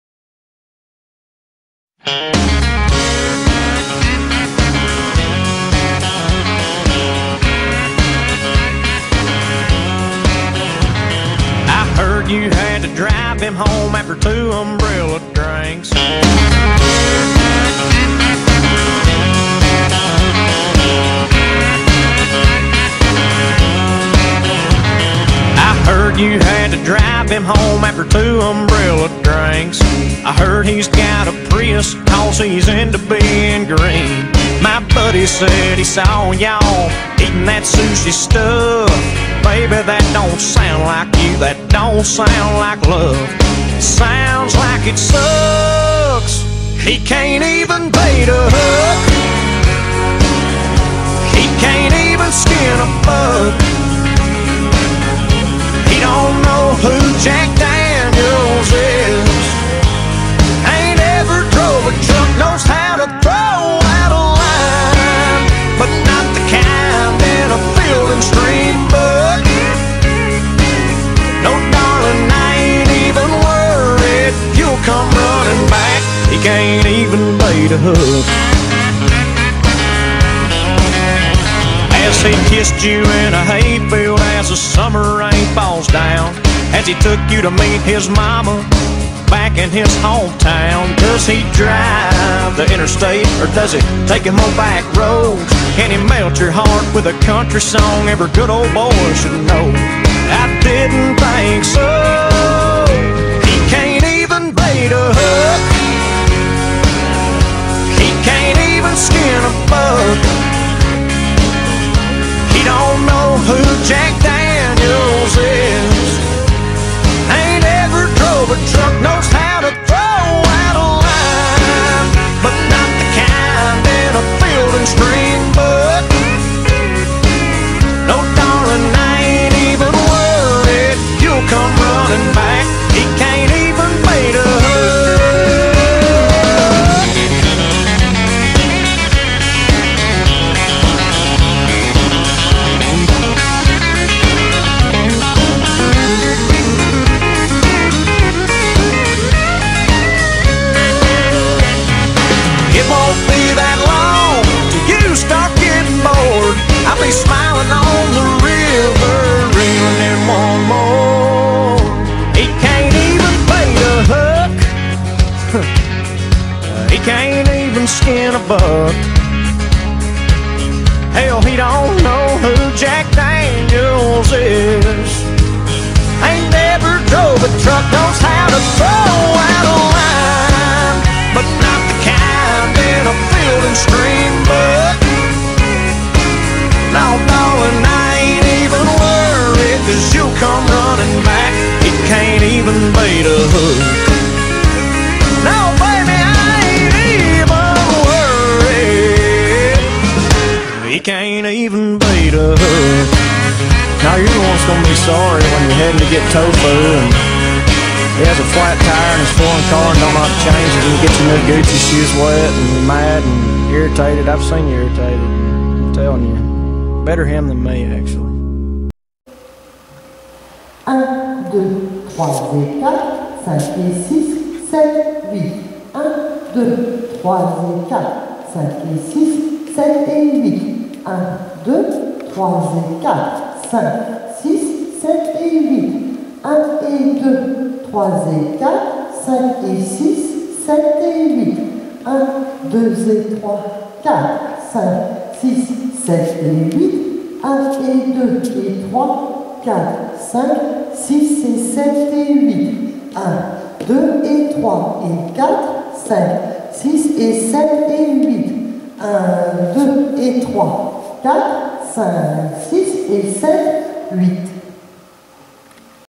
FISHING FOR LOVE_line dance
chorégraphie de Chrystel Durand (mars 2012)
danse country en ligne _64 temps_niveau intermédiaire